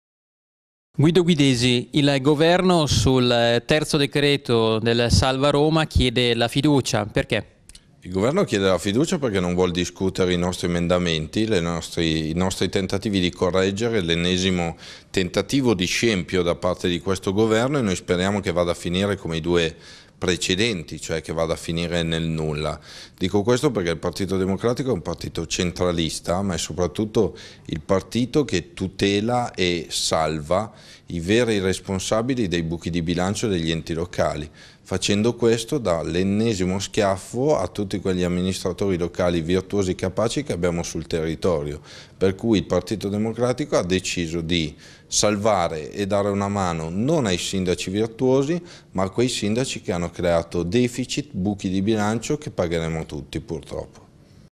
SALVA ROMA TER: GUIDESI (LN), DA GOVERNO FIDUCIA PER NASCONDERE E SALVARE VERGOGNOSE AMMINISTRAZIONI
ROMA, 9 APRILE -- " Con la fiducia Renzi si rifiuta di discutere i nostri emendamenti che avrebbero ristabilito un minimo di giustizia condannando chi amministra male e premiando chi lo fa con onestà e lungimiranza. Ancora una volta il governo, con il terzo salva Roma, aiuta e assolve chi spreca e schiaffeggia invece gli enti virtuosi. Con l'ennesima fiducia di un esecutivo che aveva garantito che ne avrebbe fatto a meno, tentano di occultare i soliti favori a Roma e a Napoli. Nonostante il suo ruolo passato da sindaco, il premier riconosce solo i  suoi omologhi di partito del tutto inadeguati a governare e nulla fa per chi invece con serietà e impegno riesce a gestire tra mille difficoltà gli enti locali . Il partito democratico in questo provvedimento si dimostra per quello che è, centralista, assolutore di chi ha amministrato male e ha buttato senza alcun rispetto i soldi dei cittadini. La sinistra che governa il paese salverà i responsabili della mala gestione. Solo i soliti impuniti saranno grati a Matteo Renzi, non certo i cittadini e gli amministratori onesti che sborseranno ancora una volta di tasca propria per riparare gli errori degli altri".

Lo dichiara Guido Guidesi, componente della commissione bilancio per la Lega Nord.